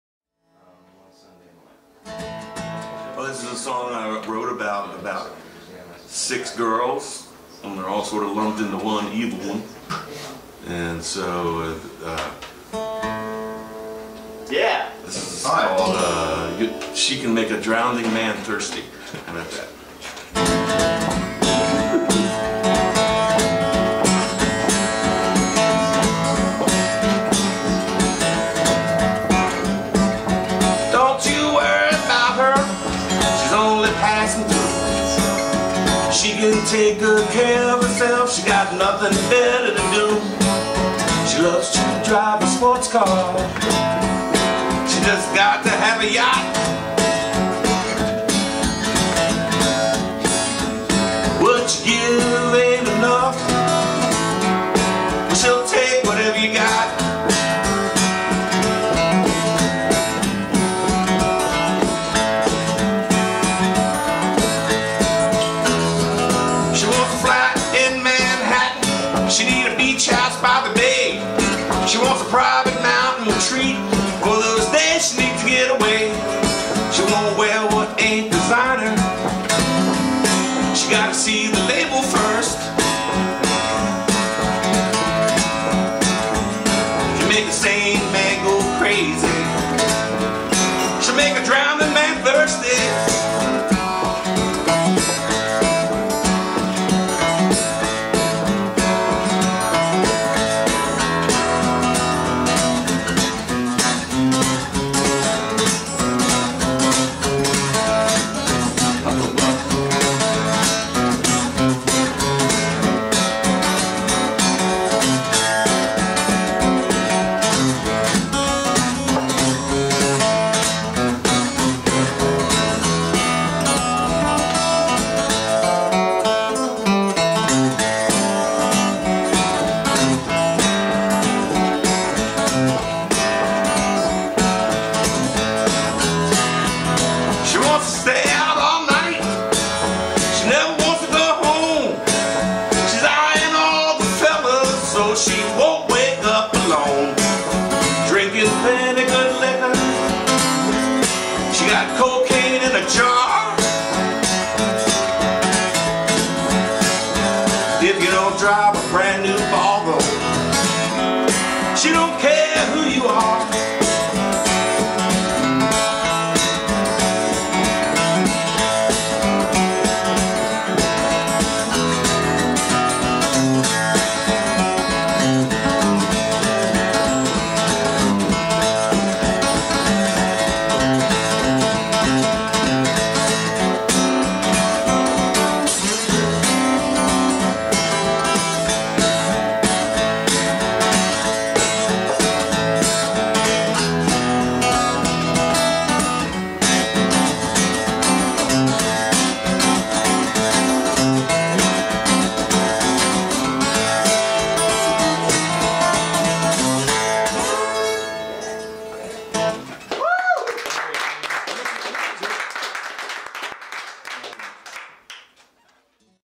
Dean Rosenthal
Dean, playing at Alexander Peters' and Skribe's Songwriter Series, at 49 West Coffeehouse, Winebar & Gallery. Filmed by Naptownmusic on 10.6.14